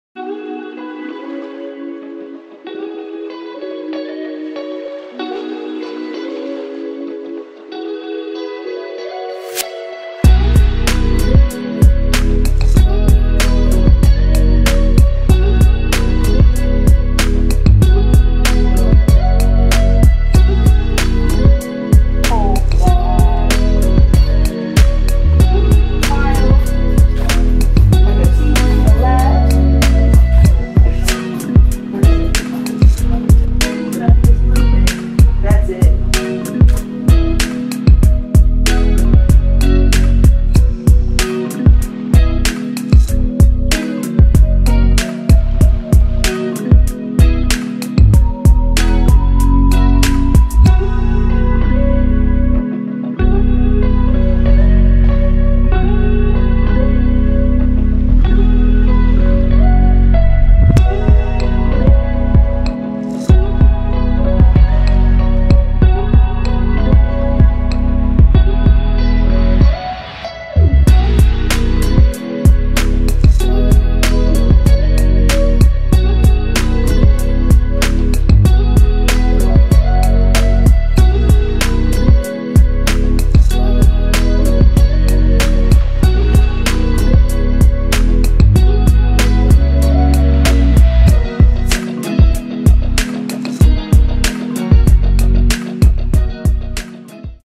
Melody's Boudoir Shoot / Glam Brand Photography / Stockton, CA
Behind the scenes video from Melody's multiple look boudoir photoshoot, featuring the NEW bedroom set at Glam Brand Photography in Stockton, CA.